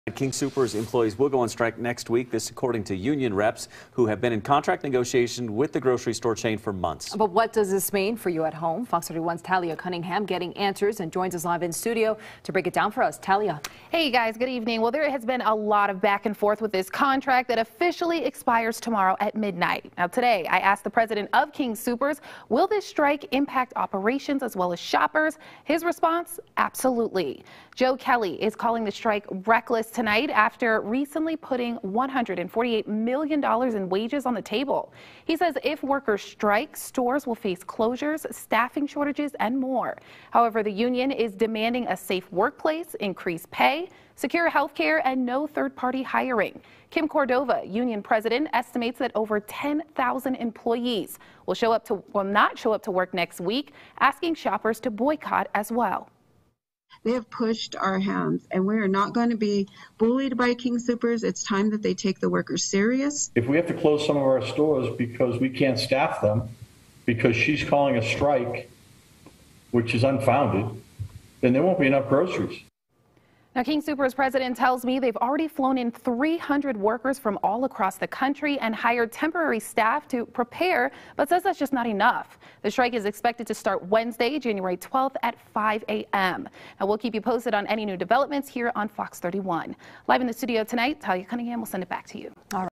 King Soopers workers to strike
King Soopers employees will go on strike next week, according to union representatives who have been in contract negotiations with Colorado's largest grocer for months. Talya Cunningham reports.